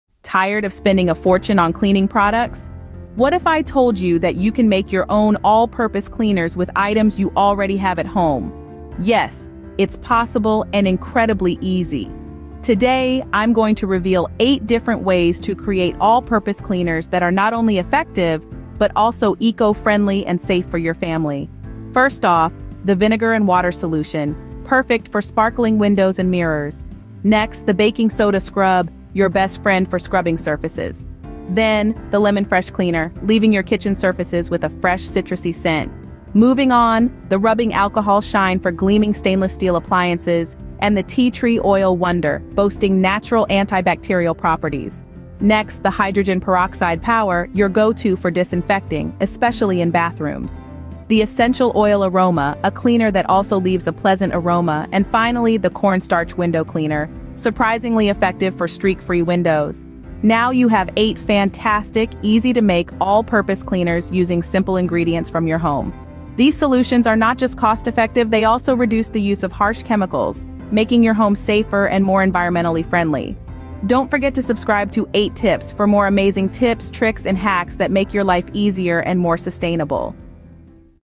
Escape Harmful Chemicals with These 8 Genius Homemade Cleaners
In this video, we'll be demonstrating how to make 8 genius homemade cleaners that will help you detoxify your home of harmful chemicals. From natural cleaning solutions to toxic-removal pads, these cleaners are perfect for anyone looking to reduce their exposure to harmful chemicals.